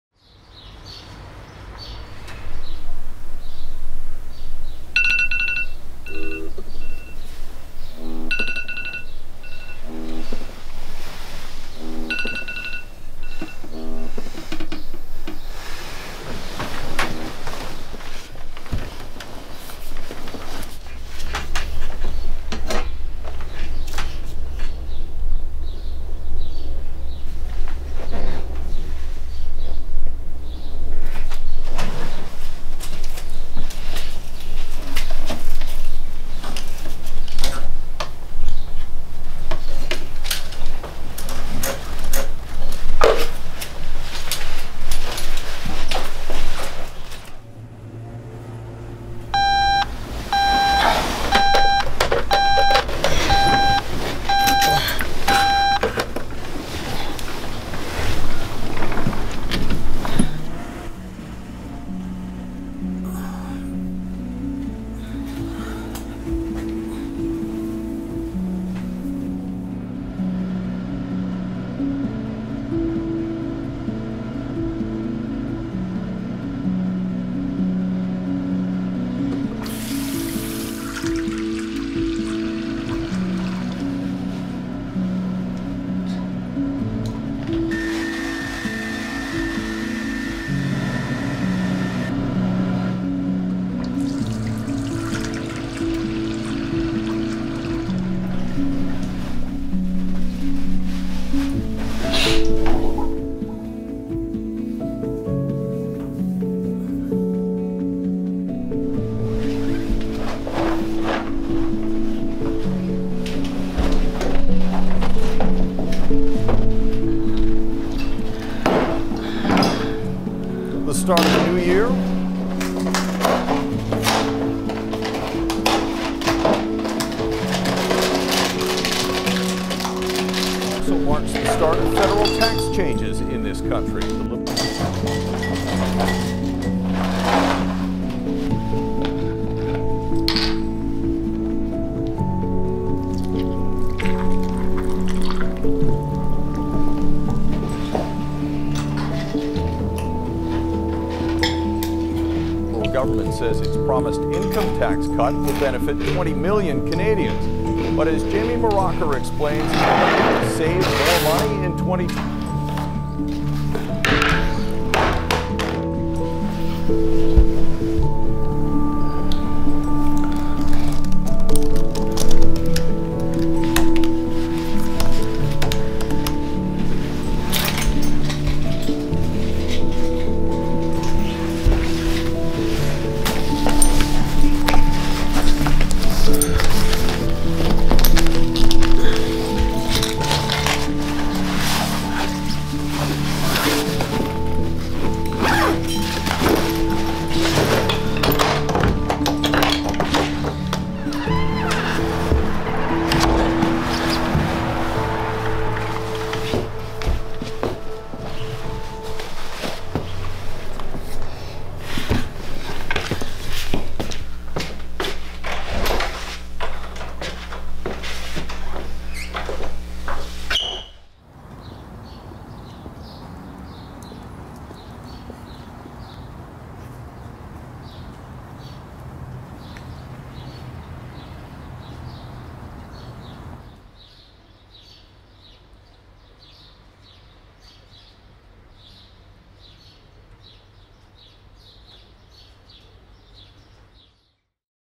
Fuji - Inequality Short Film
A contrasting story about economic inequalities.

Filmed and Edited by Austin Tong.